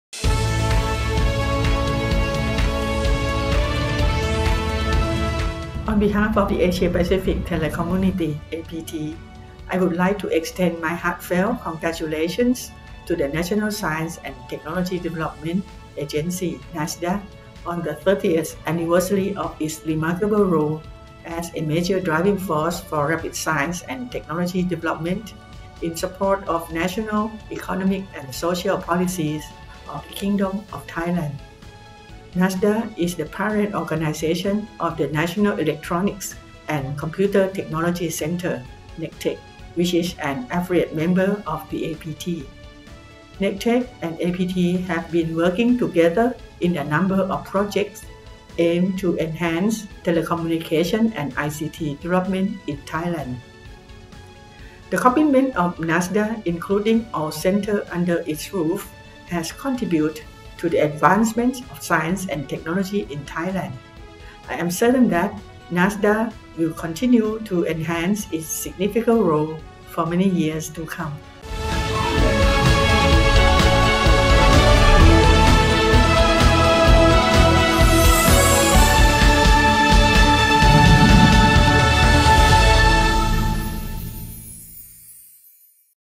30th NSTDA Anniversary: Congratulatory Message from Asia-Pacific Telecommunity (APT)
Congratulatory Message of Ms. Areewan Haorangsi, Secretary General, Asia-Pacific Telecommunity (APT)

“ขอแสดงความยินดีอย่างจริงใจในโอกาสครบรอบ 30 ปี สวทช. ที่ได้มีบทบาทสำคัญในการขับเคลื่อนพัฒนาการทางวิทยาศาสตร์ และเทคโนโลยีอย่างก้าวกระโดด ซึ่งเนคเทค สวทช. สมาชิกในเครือข่าย APT ได้ทำงานร่วมกันในหลายโครงการ สร้างคุณประโยชน์ให้กับความก้าวหน้าทางวิทยาศาสตร์และเทคโนโลยีของประเทศไทย ซึ่งจะมีบทบาทสำคัญมากขึ้นต่อไปในอนาคต”